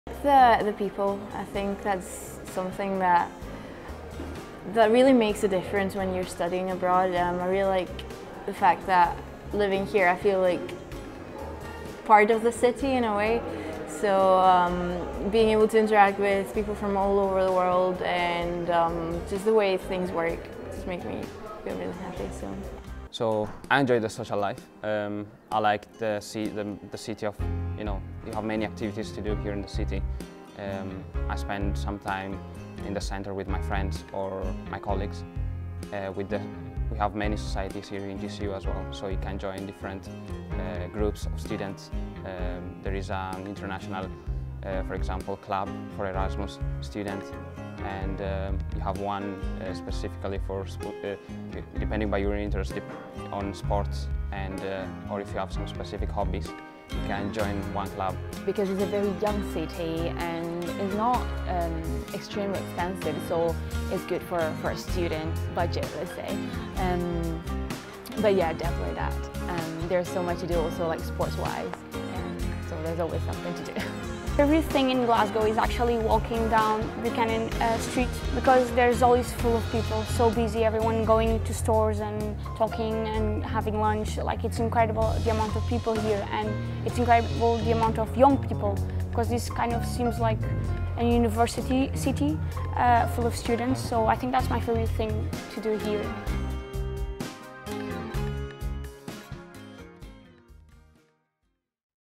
GCU students from the European Union
Learn about the GCU experience from some of our European students.